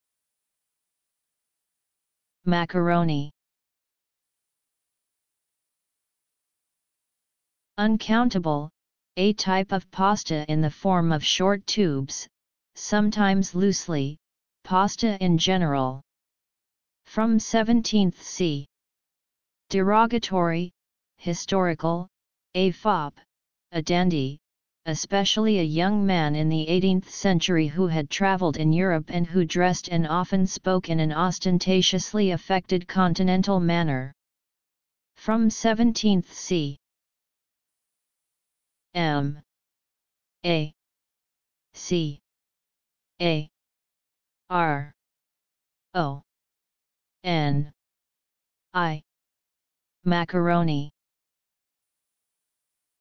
what is the meaning of macaroni.
what macaroni means:
(uncountable) A type of pasta in the form of short tubes; sometimes loosely, pasta in general. [from 17th c.] (derogatory, historical) A fop, a dandy; especially a young man in the 18th century who had travelled in Europe and who dressed and often spoke in an ostentatiously affected Continental manner. [from 17th c.]